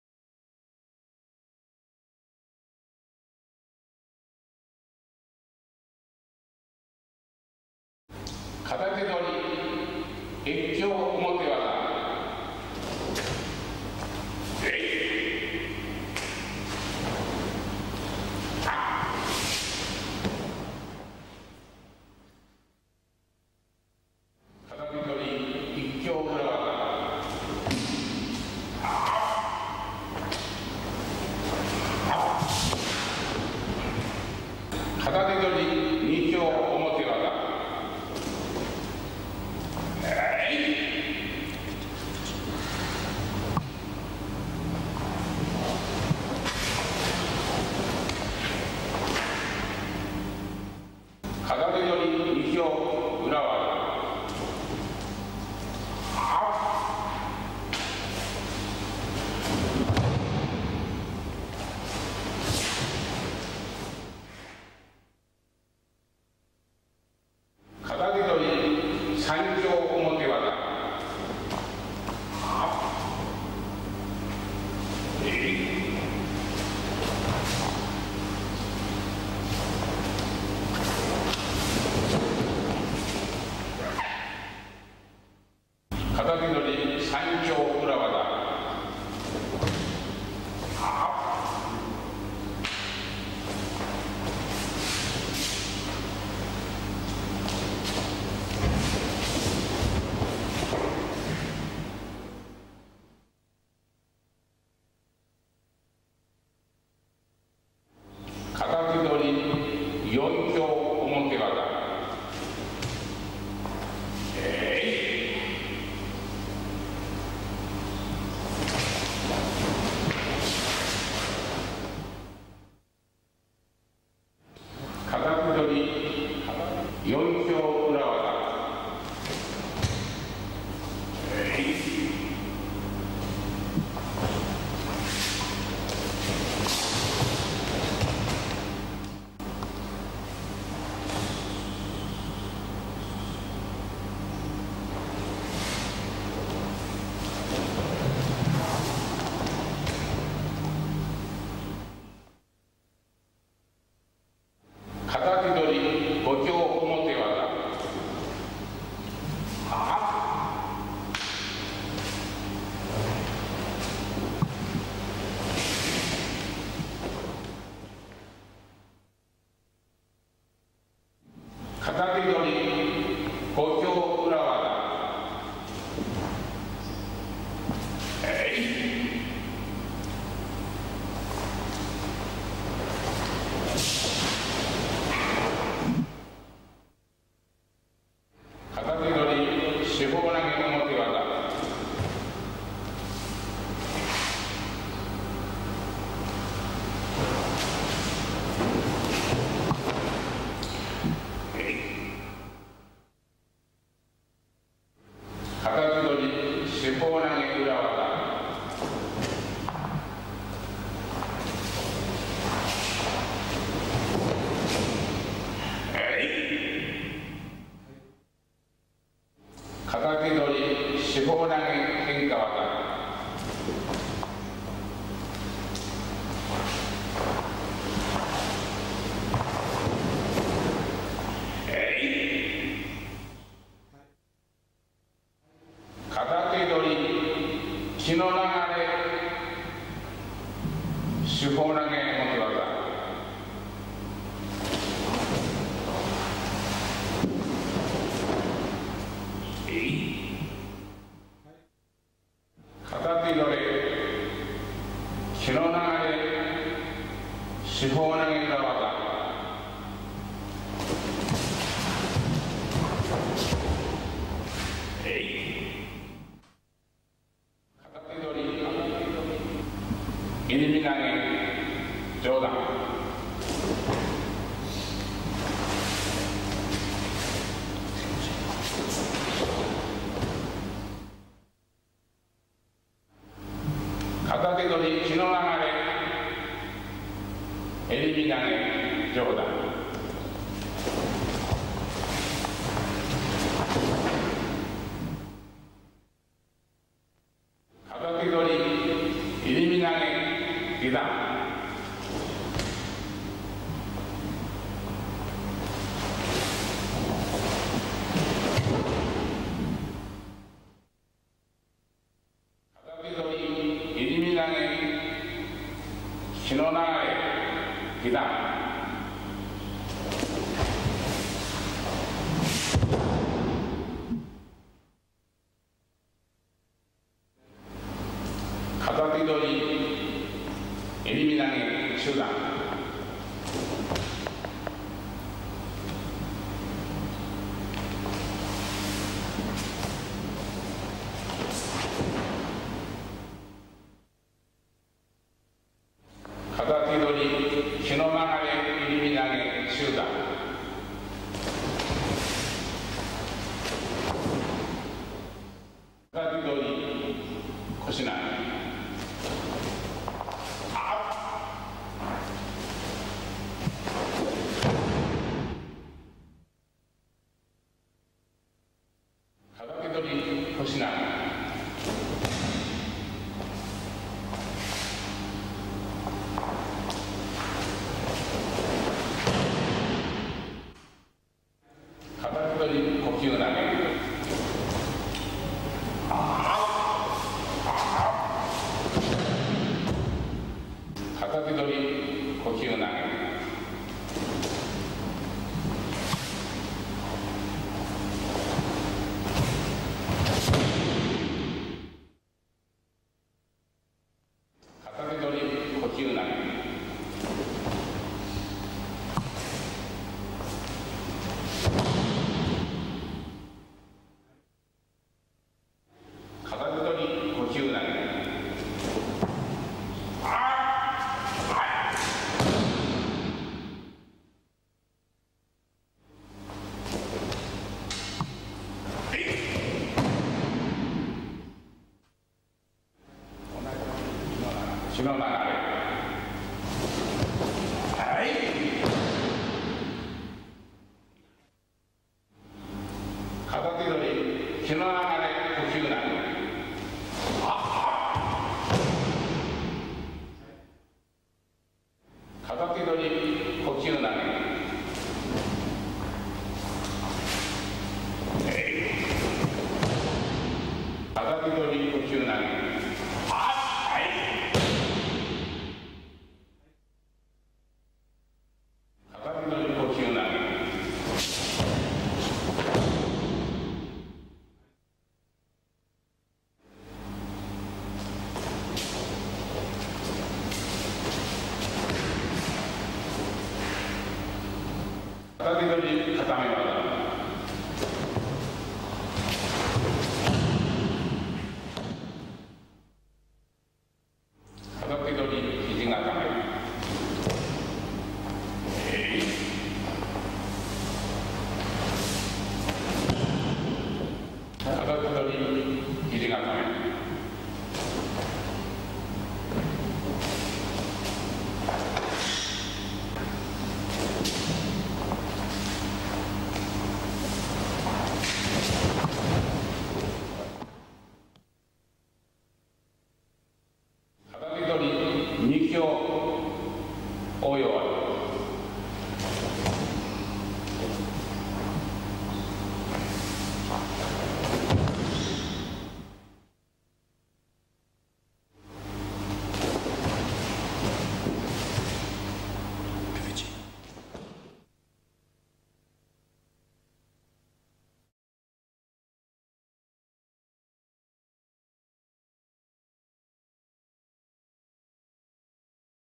Katate Dori - Morihiro Saito Sensei.
Morihiro Saito Shihan performing basic techniques from Katate Dori on uke Bjorn Saw. Filmed in Jesi, Italy 1992. Produced by Paolo Corallini Shihan.